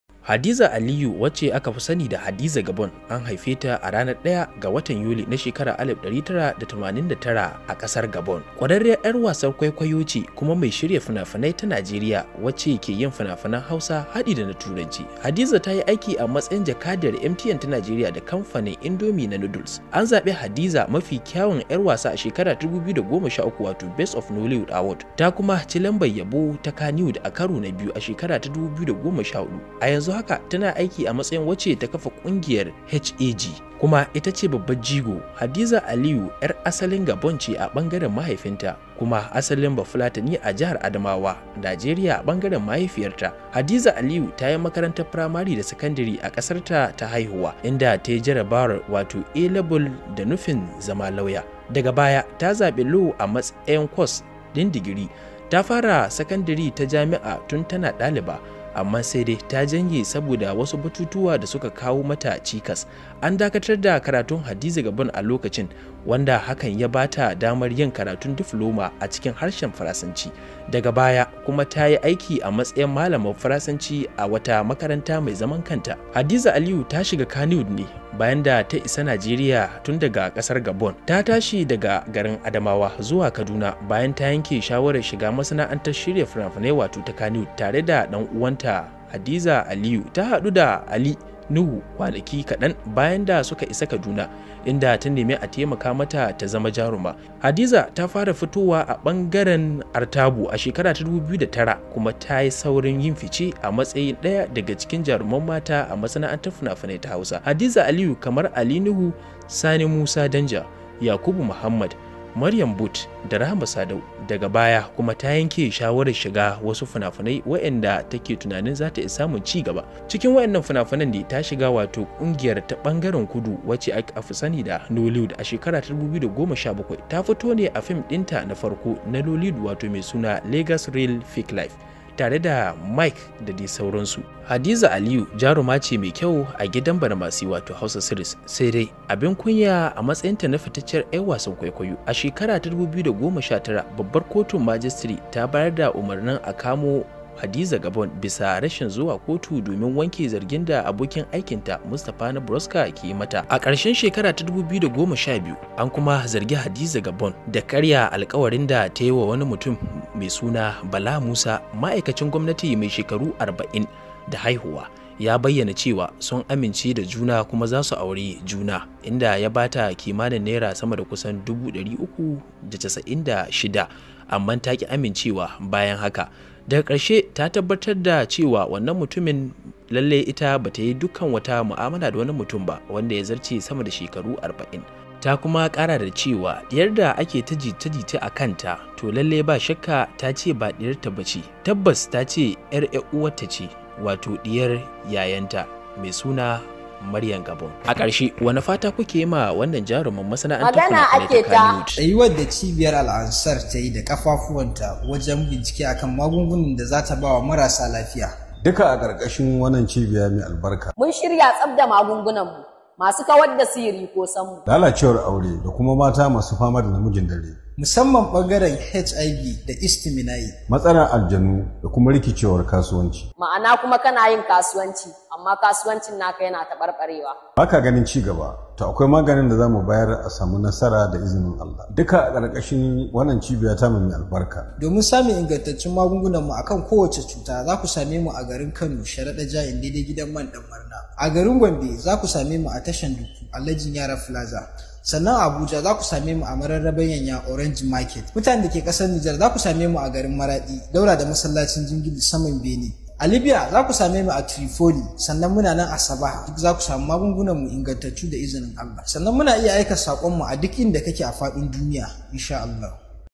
HADIZA GABON:Abubuwan da yaka mata kusani akan jaruma hadiza aliyu gabon cikekken tarihinta 2024
Hadiza Aliyu Gabon who is also known as “Hadiza Gabon” is a Nigerian actress and filmmaker who acts in both Hausa (kannywood) and English (Nollywood) movies.

 / @Arewafirm tv
Hadiza Gabon also served as an ambassador to MTN Nigeria and indomie noodles company,she is an award winning actress who has won numerous awards home and abroad,one which is the 2013 BEST OF NOLLYWOOD AWARDS for “BEST ACTRESS “